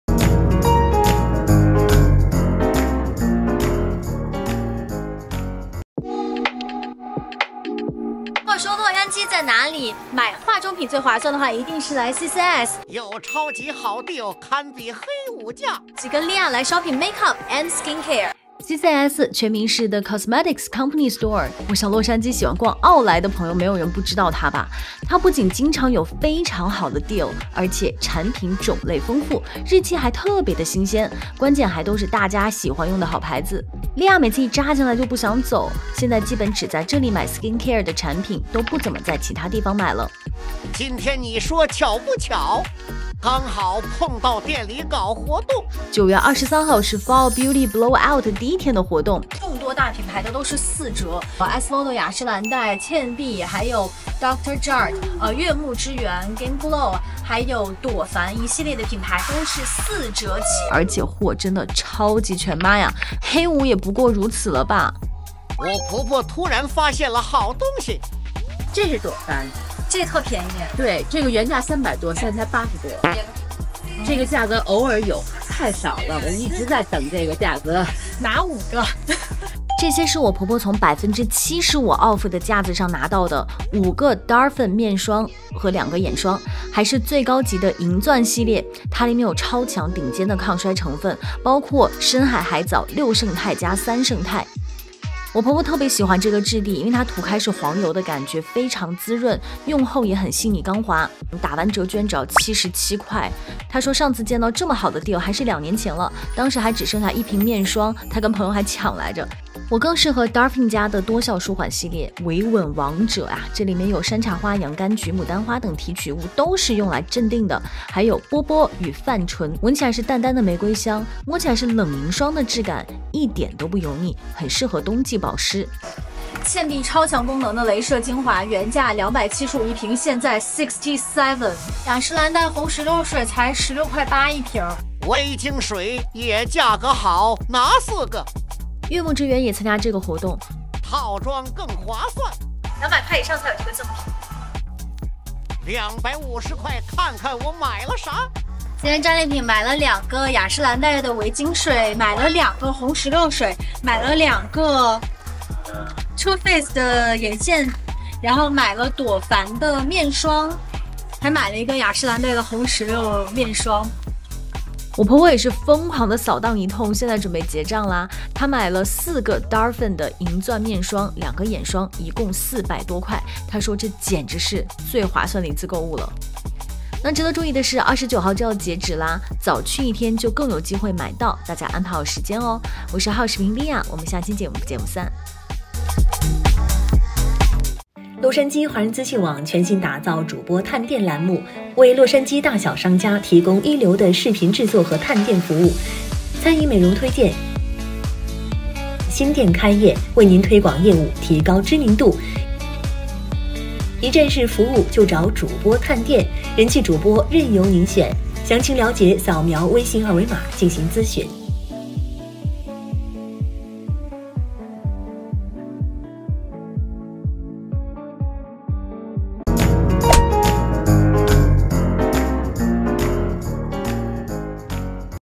2024年黑五提前来了！奥莱超级好价；CCS大牌护肤品四折起 都有啥好买？跟Leah一起逛街！主播探店 1005
the costmatic company store，十个门和沙漠奥莱都有！